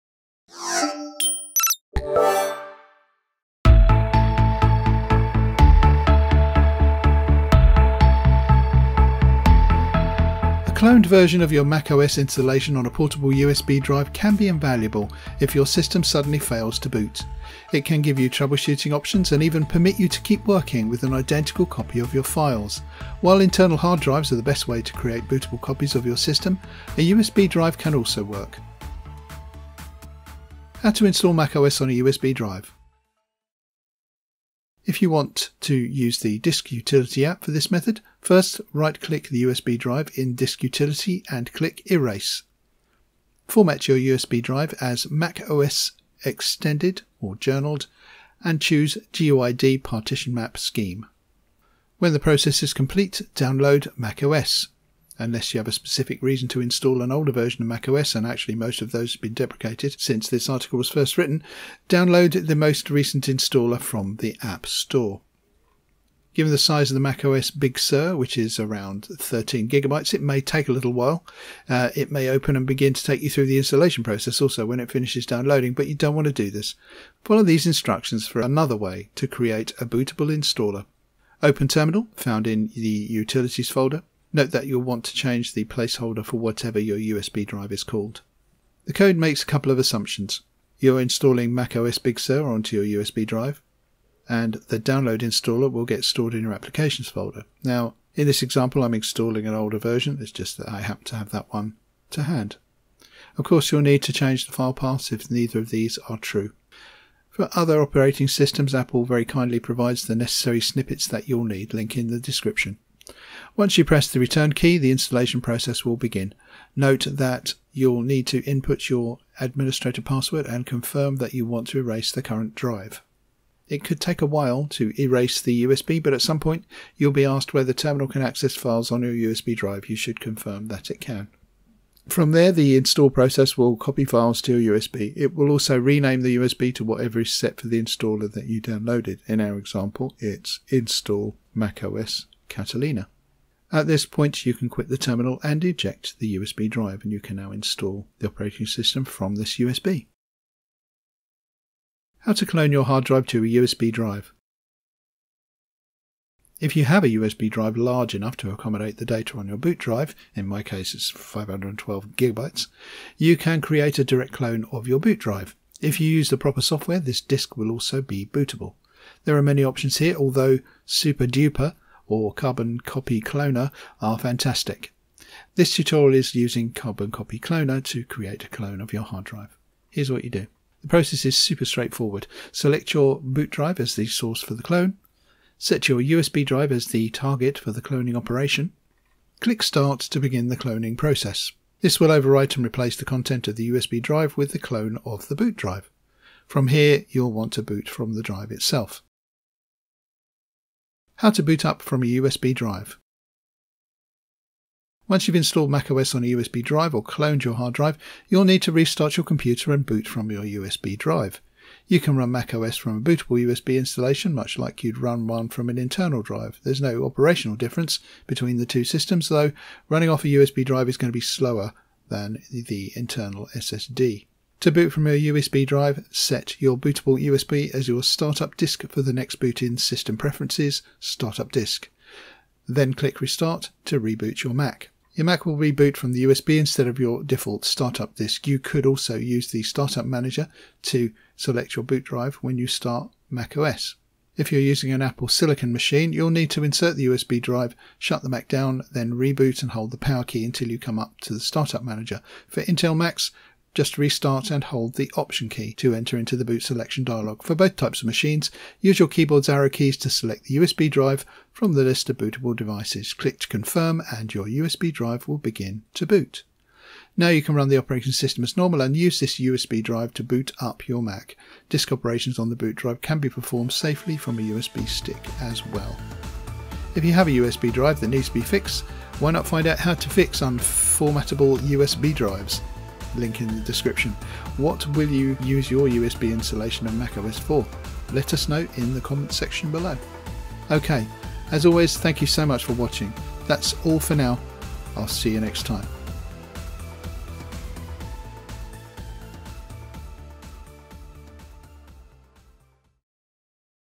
How to Boot macOS from a USB
What's that? macOS on a USB drive? Yes, we truly have arrived in the future if you can wear the slick OS around your neck like a Macolyte Medallion. Here we're going to show you how to install macOS on a USB drive, cloning your entire hard drive to a USB so you can boot it up from any other Mac you plug it into.